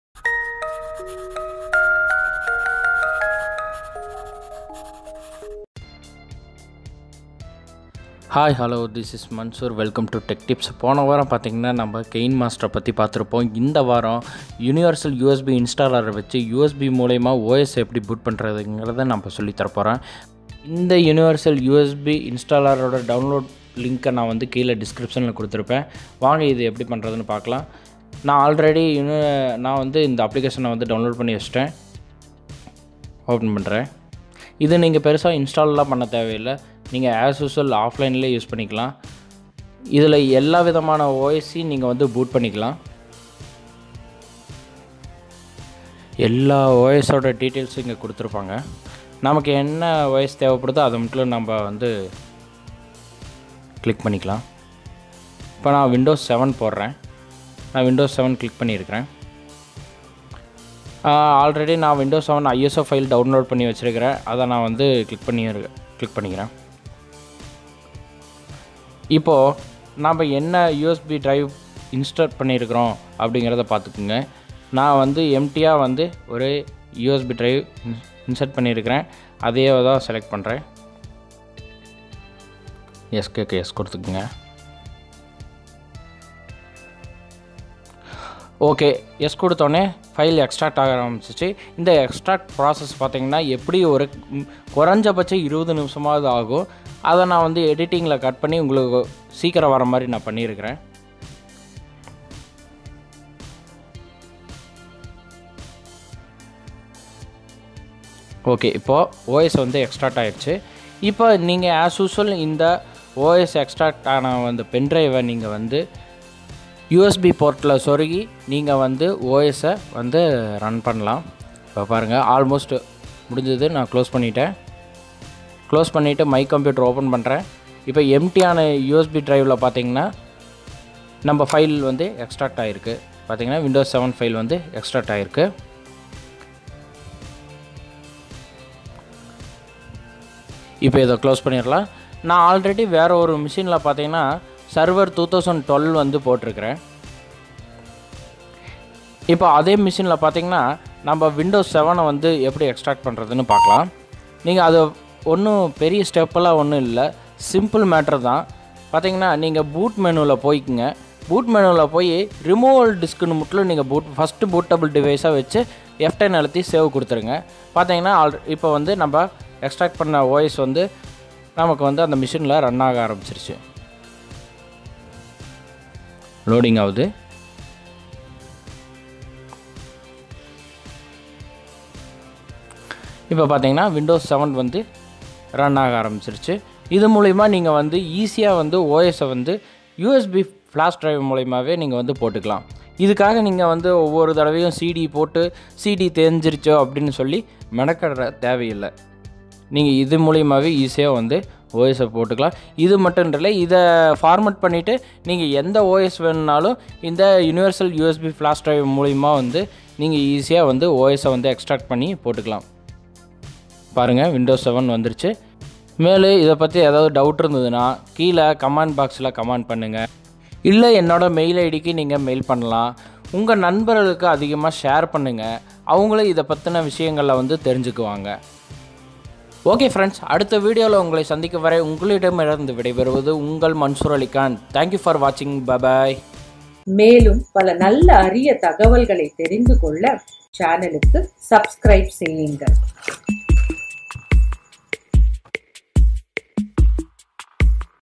How to Make bootable USB Drive with Universal USB Installer in Tamil
Universal USB Installer (UUI) is a Live Linux USB Creation tool, which lets you choose from a selection of Linux Distributions to add to a USB Flash Drive.

Universal USB Installer is easy to use; simply choose a Live Linux Distribution, select the ISO file, and your Flash Drive and then click Install. Upon completion, you will have a bootable USB Flash Drive with your selected operating system installed, ready to run.